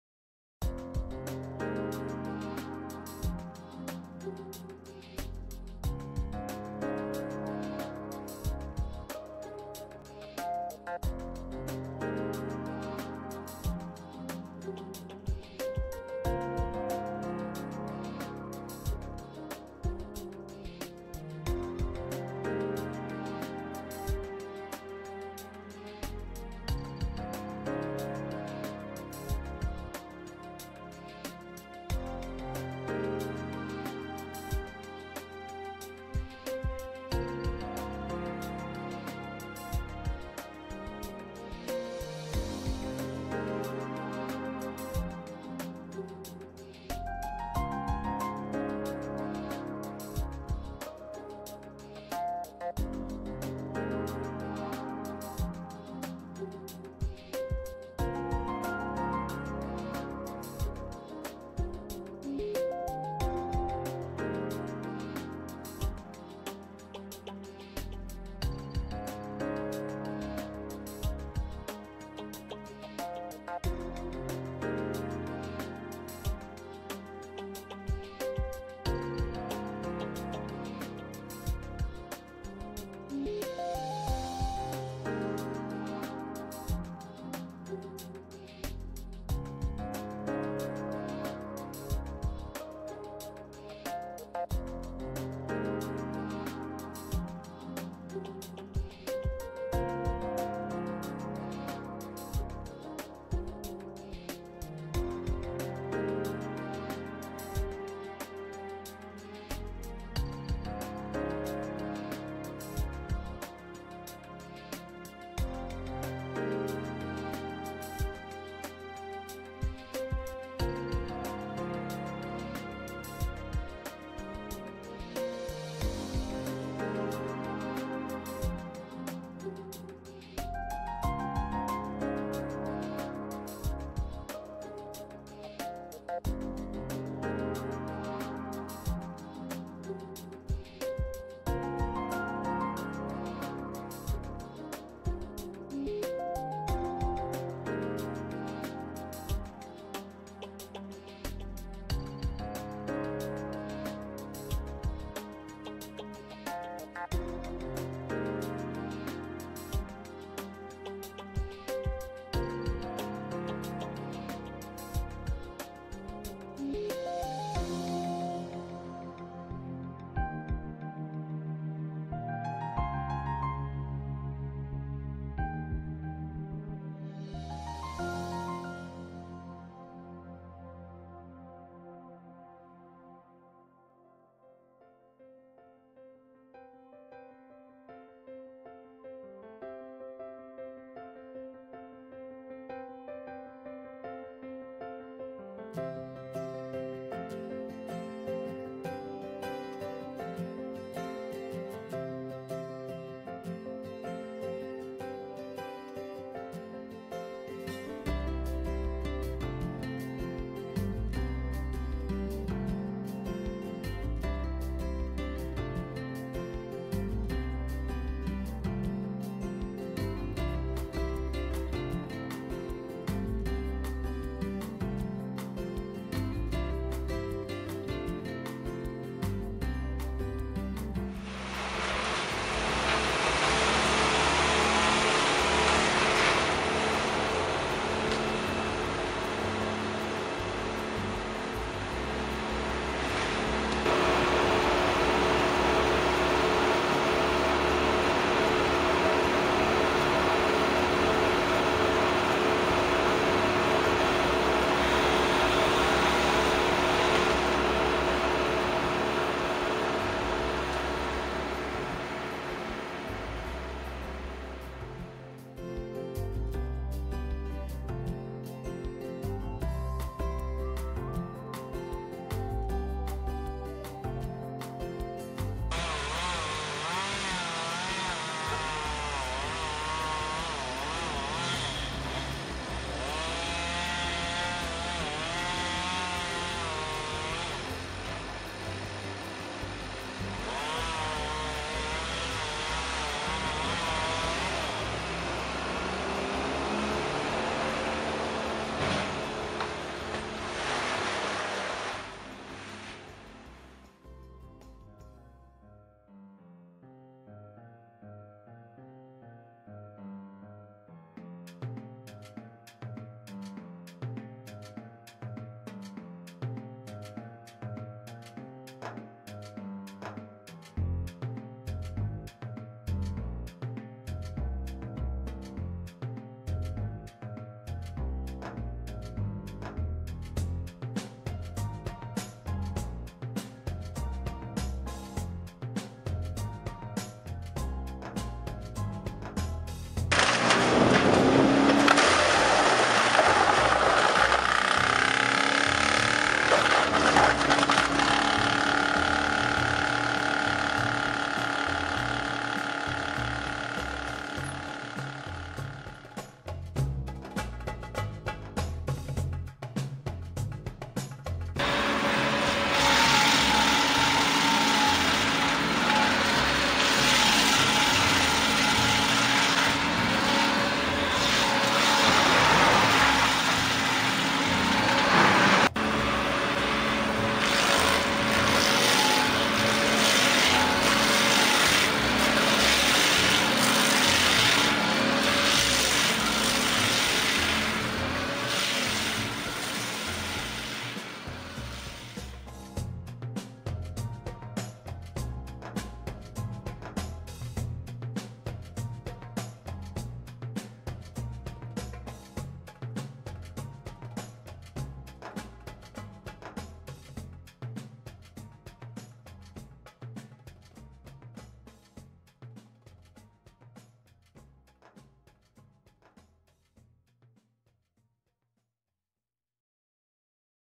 Tree Removal
How to remove a giant tree on your property.